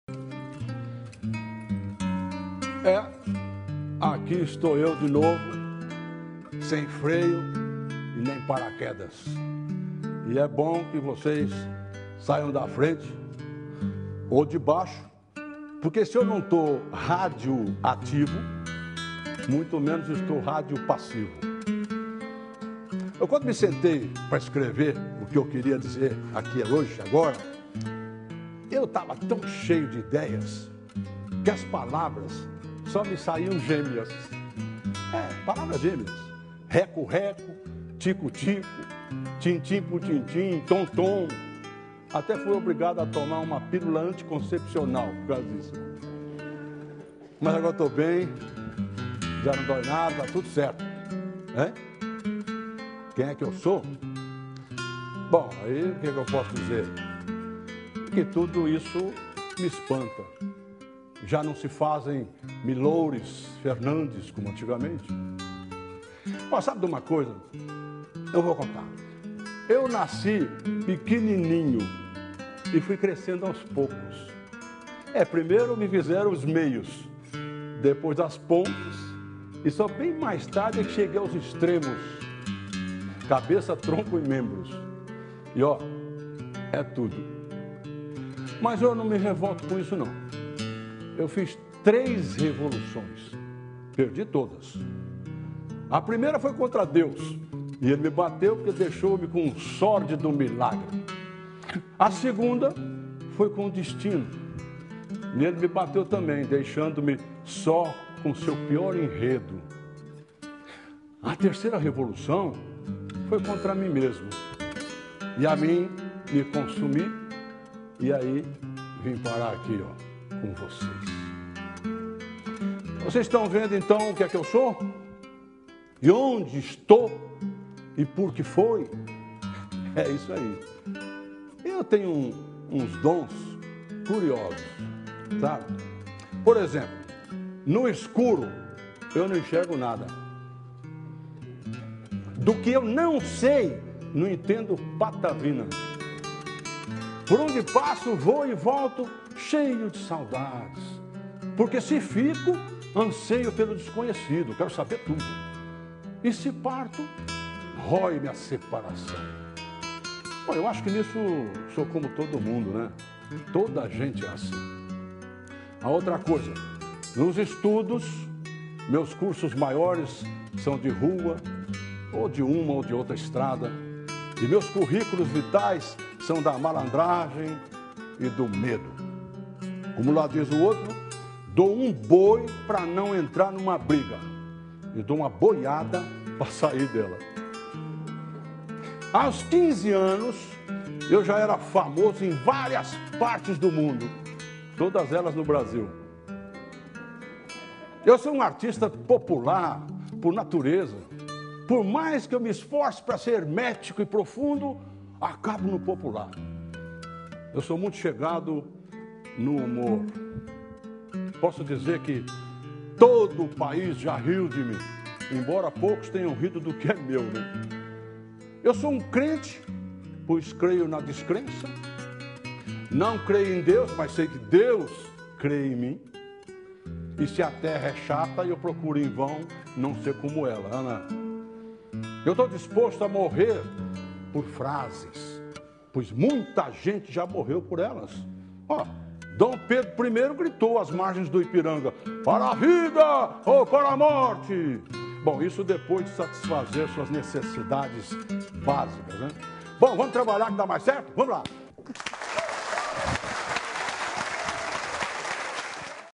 ''Autobiografia de mim mesmo, à maneira de mim próprio'', por Rolando Boldrin
Rolando Boldrin recita texto de Millôr Fernandes em seu programa.